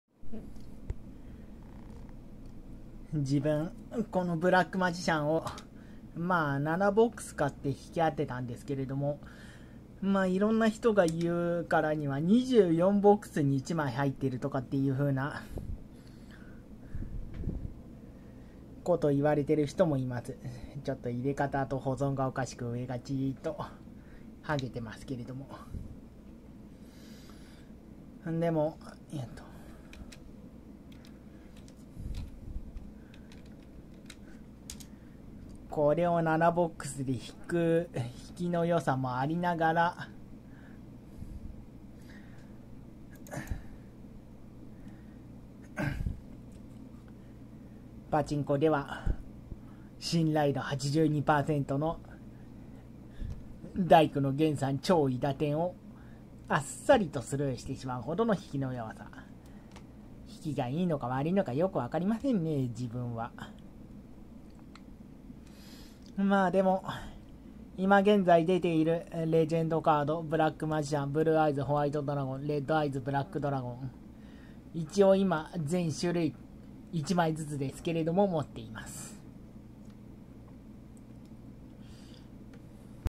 レジェンドレアのブラックマジシャンを引けても
パチンコの82％の信頼度は簡単に外します。　遊戯王　レジェンドレア　みさわ